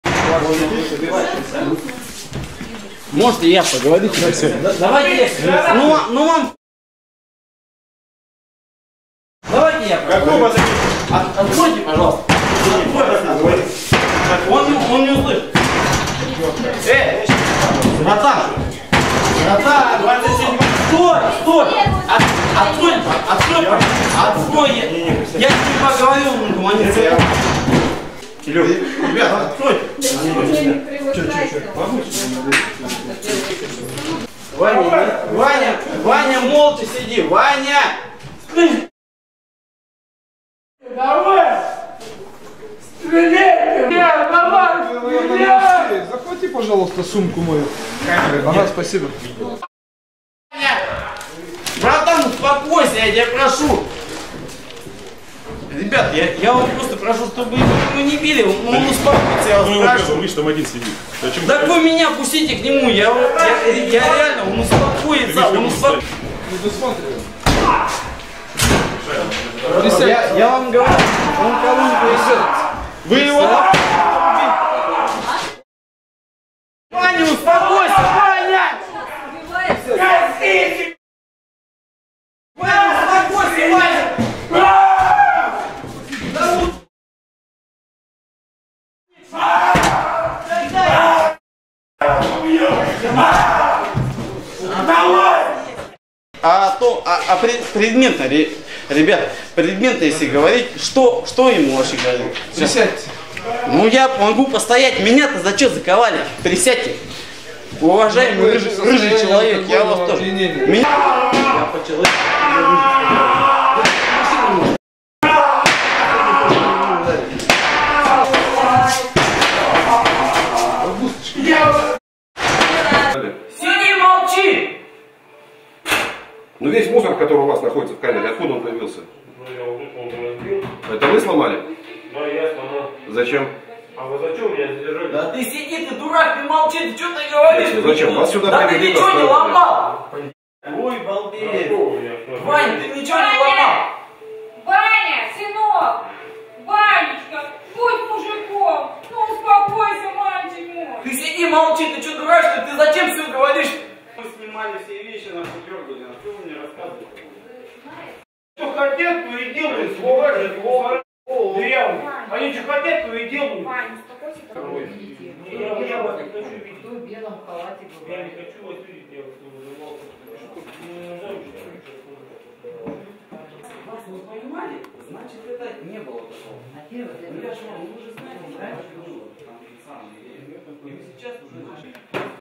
Дебош в дежурной части аэропорта «Толмачёво»
Видео предоставлено пресс-службой Управления на транспорте МВД России по СФО